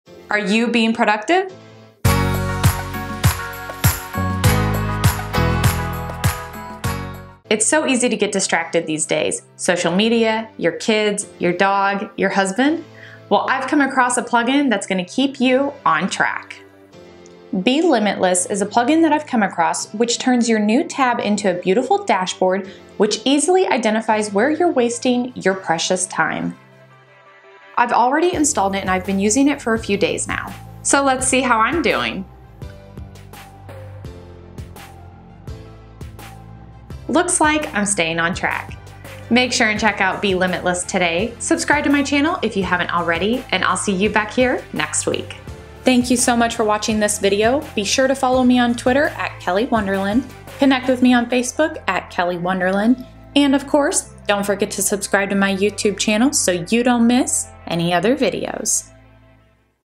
Track where you go online using Google Chrome
Be Limitless chrome extension replaces your new tab to manage your productivity